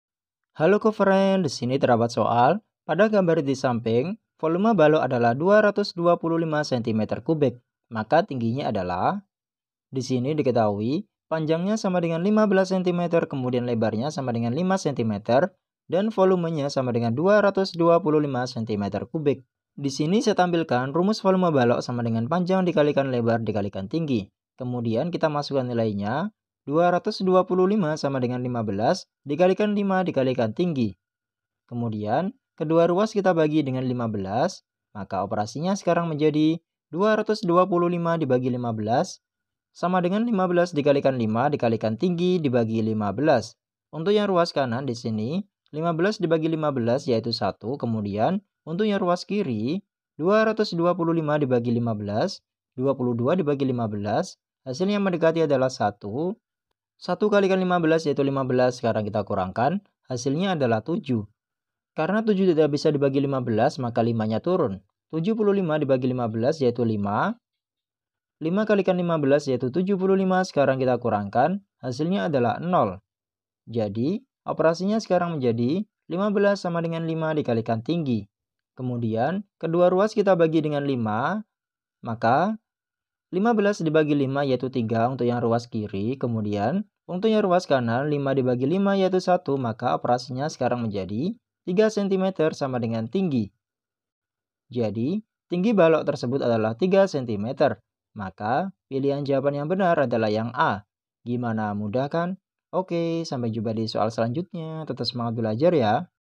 Pada gambar disamping, volume balok 225 cm? maka tingginya adalah I! ! " a.3 cm b. 4 cm IS m C. 5...
Pada gambar disamping, volume balok 225 cm? maka tingginya adalah I! ! " a.3 cm b. 4 cm IS m C. 5 cm d. 6 cm

Topik atau Materi: Volume dan Luas Permukaan Balok - Bangun Ruang - Geometri - Matematika Kelas 5 SD -

Sumber Soal: Questions from WA Business (B005) & QA User , N/A

00:00 : Bedah Soal
01:00 : Konsep, rumus dan pengertian Volume dan Luas Permukaan Balok
01:18 : Langkah penyelesaian soal
00:46 : Jawaban akhir
01:16 : Penutup